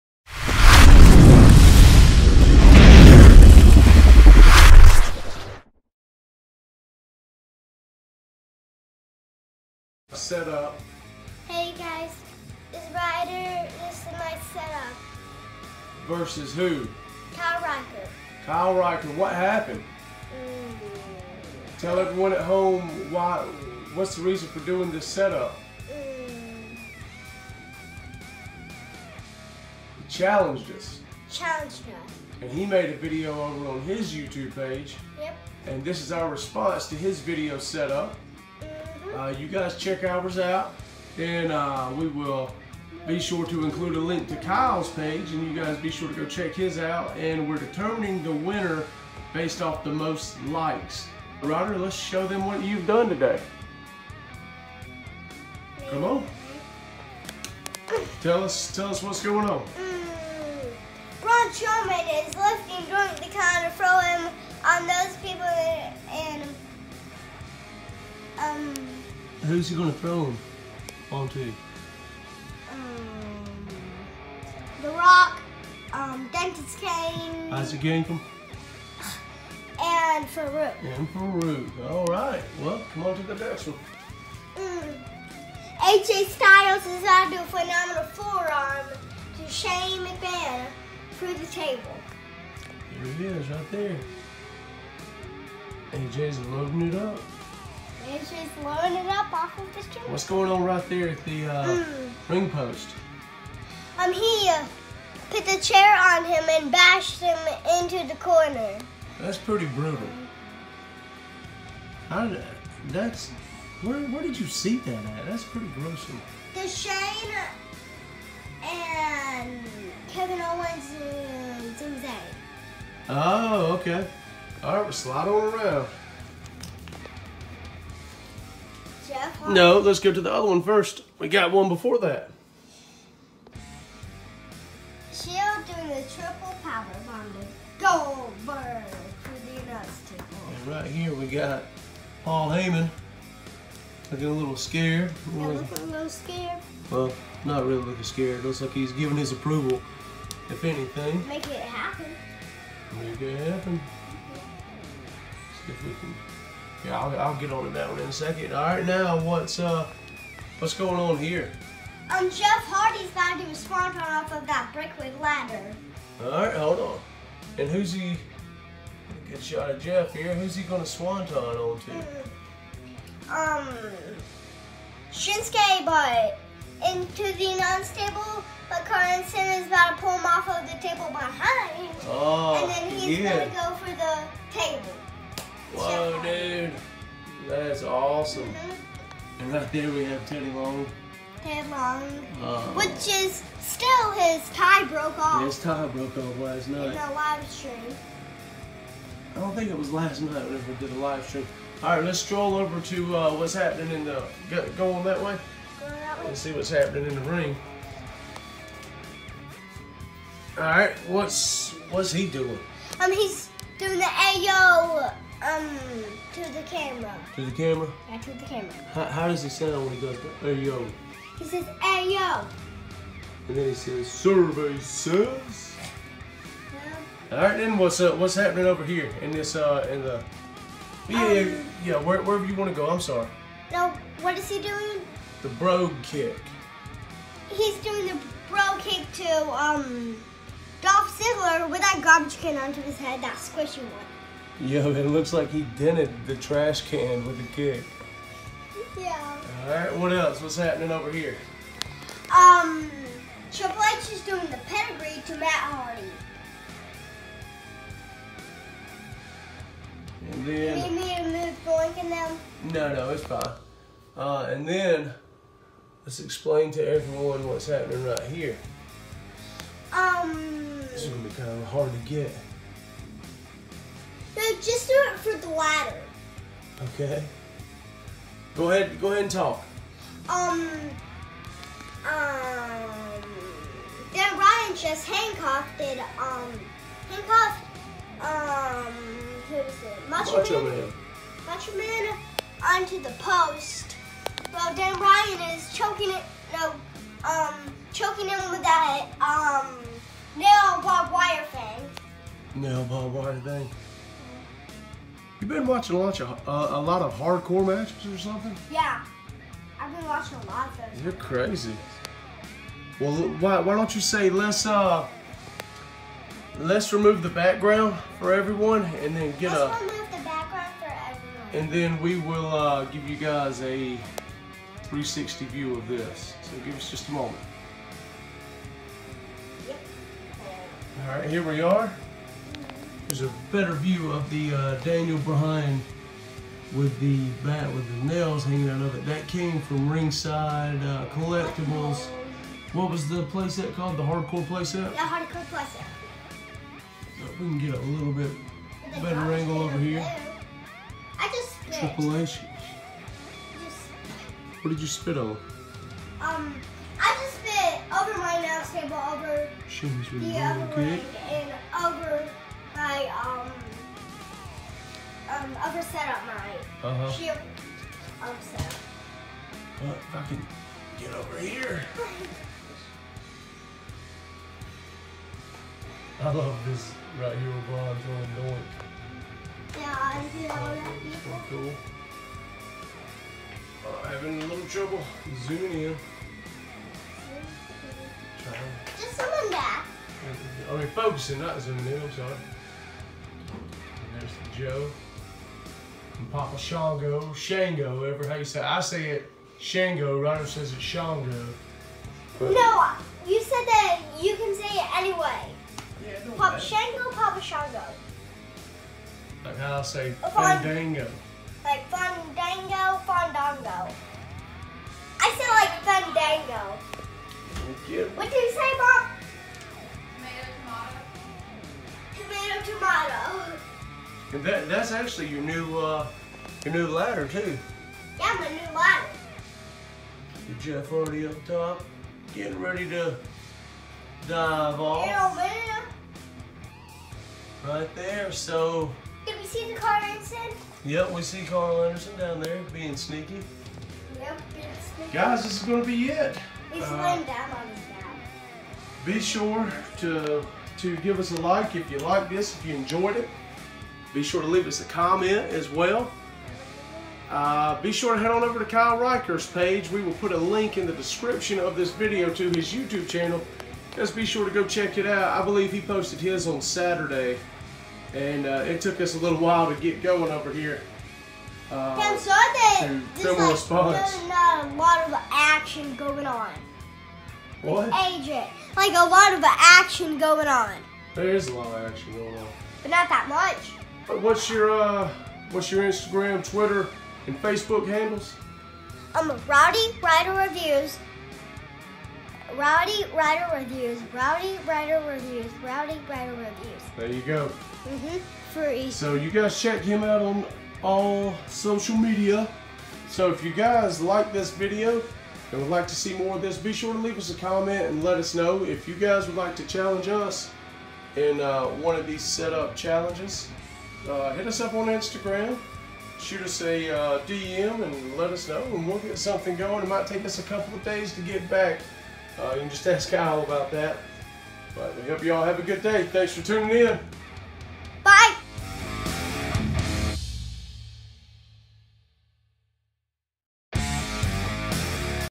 Rowdy Ryder Reviews VS Kyle Riker. WWE Action Figure Setup Collaboration.
This is our part of a WWE Setup collaboration/contest we did with our friend Kyle Riker. Be sure to go to his page, check out his videos and his part in this collaboration.

Also, make sure to put a thumbs up on whoever’s video you like better.